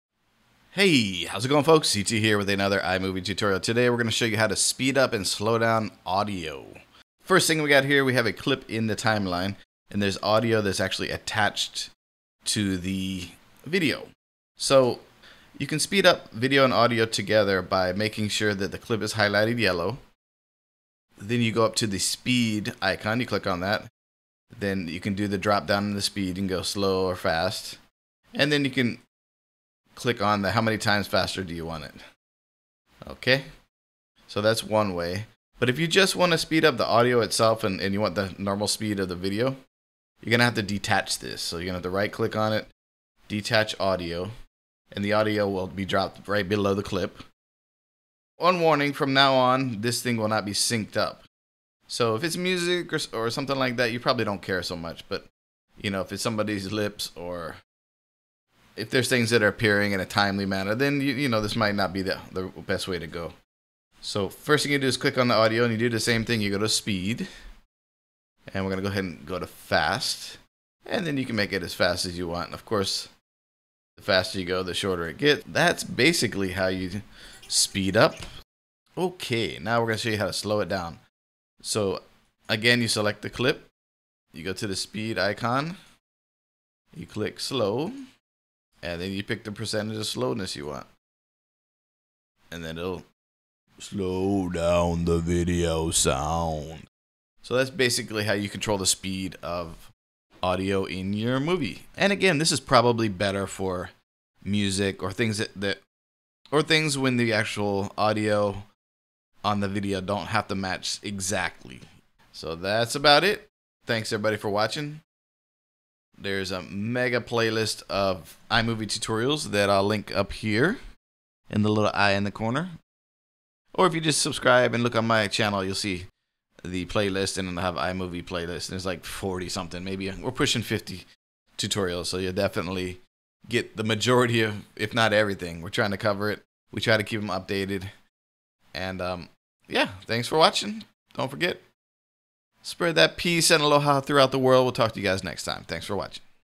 iMovie Tutorial - Speed Up Audio - Slow Down Audio
In this iMovie tutorial I show you how to slow down and speed up the audio in your movie, or a music clip / other audio clip.  ET

TECHNOLOGY USED: COMPUTER/CAMERA/SOFTWARE I AM USING?